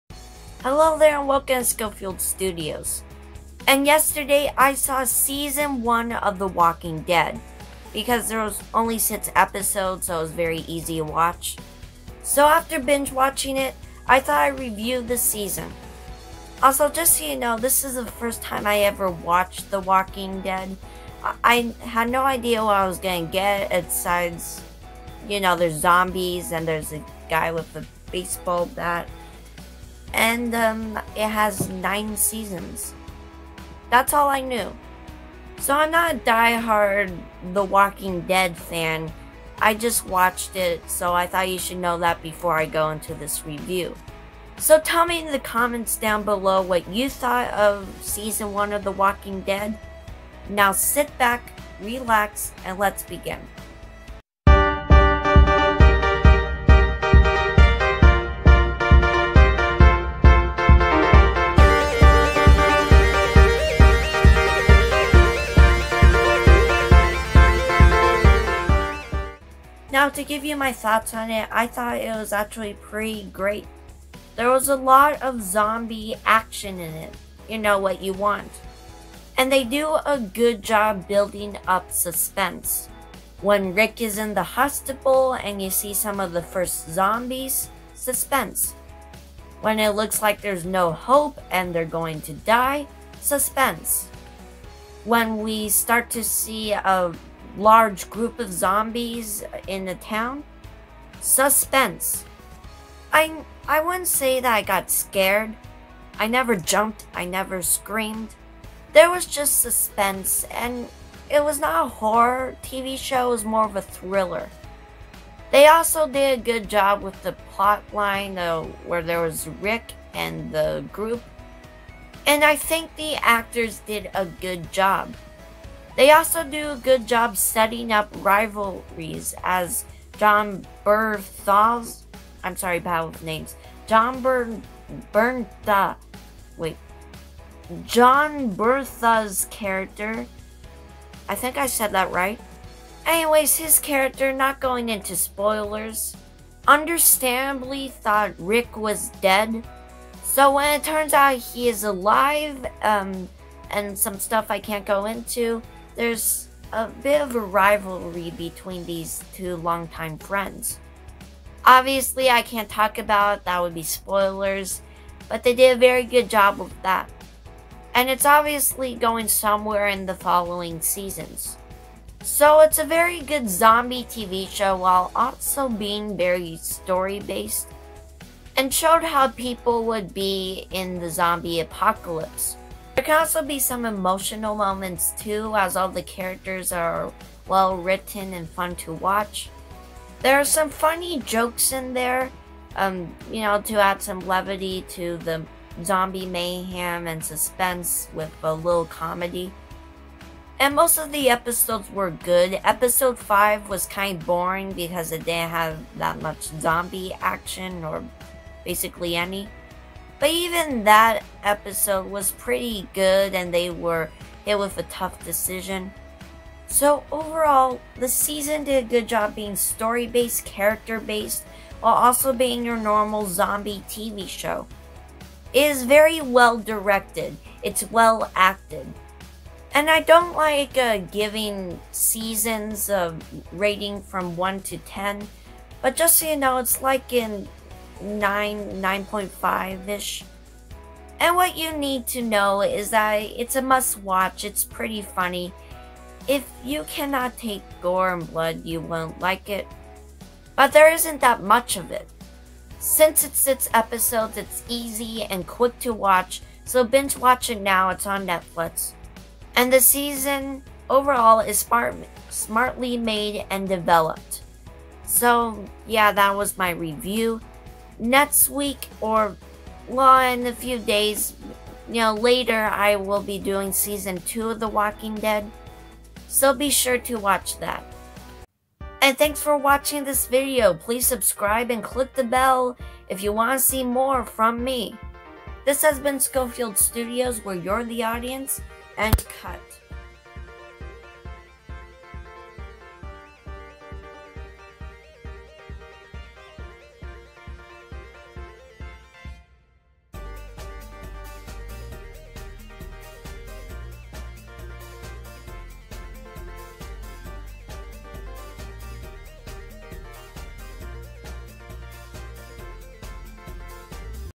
The Walking Dead Season 1 Review
I just saw TWD (The Walking Dead) season 1, so in this video I'm going to review it.